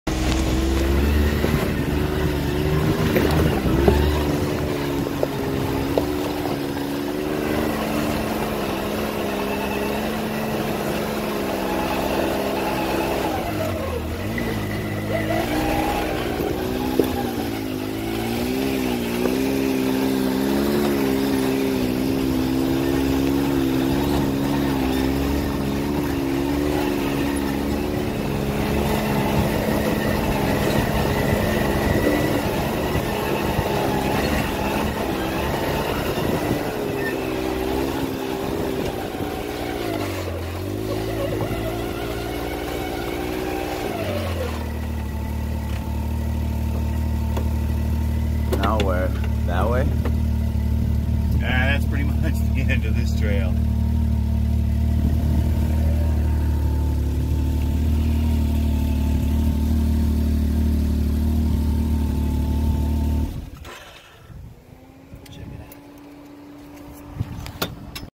Fernley, Nevada
Out on my co-workers can am side by side for the day.